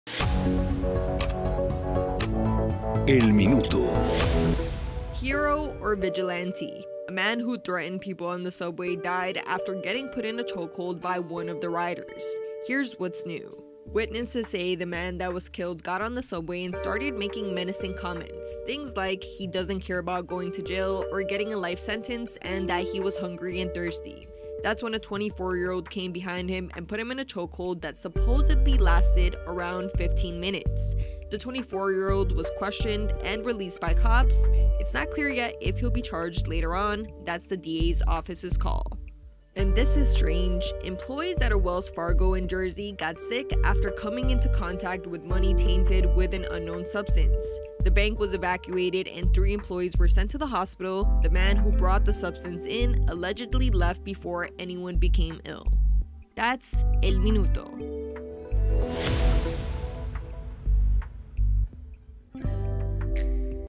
Man Who Threatened Subway Riders, Dies After Chokehold: Sources | El Minuto (English)
A 24-year-old was taken into custody for questioning and later released, the NYPD said, but the question remains: Was this heroism, or vigilantism?